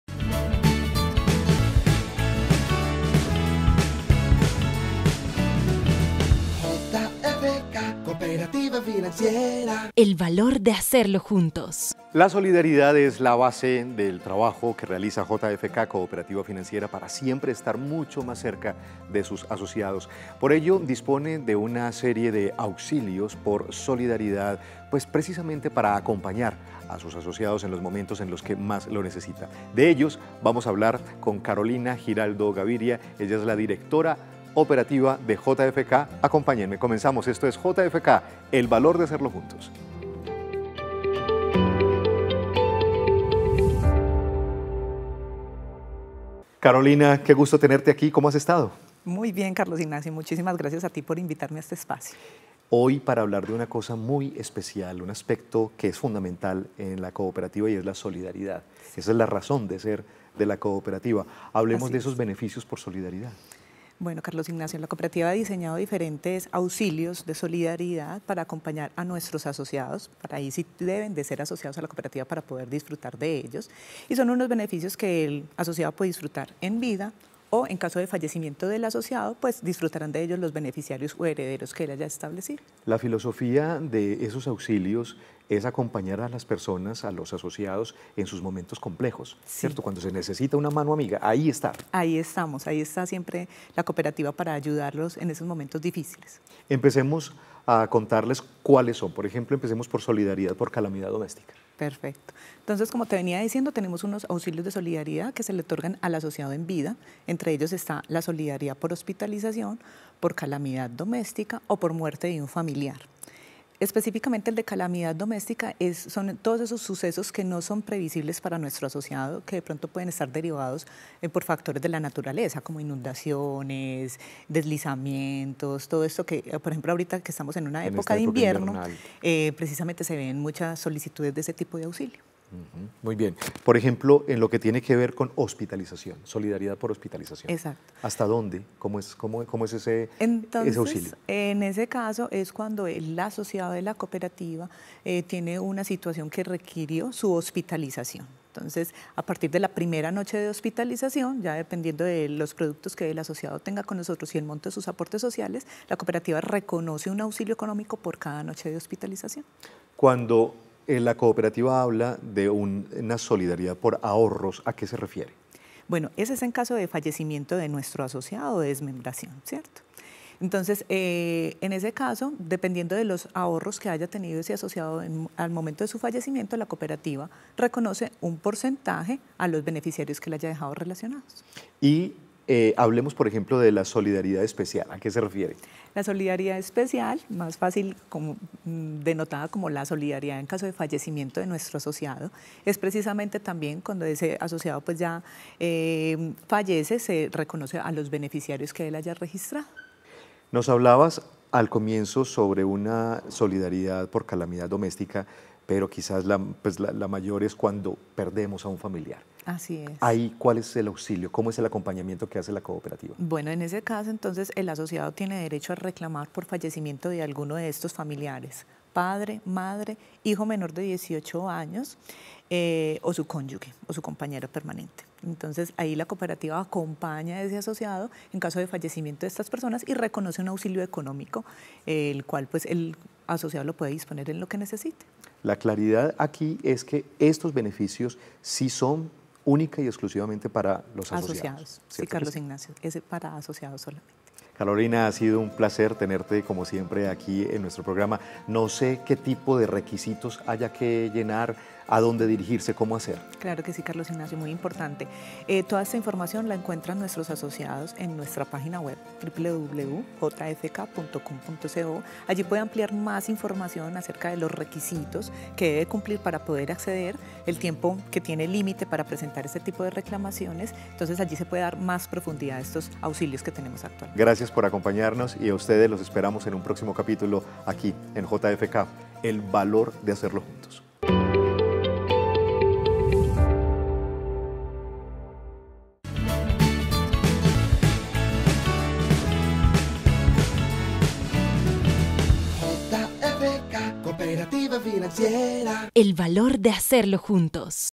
Auxilios por solidaridad | JFK
Los Auxilios por solidaridad son diseñados para acompañar a todos los asociados en esos momentos más difíciles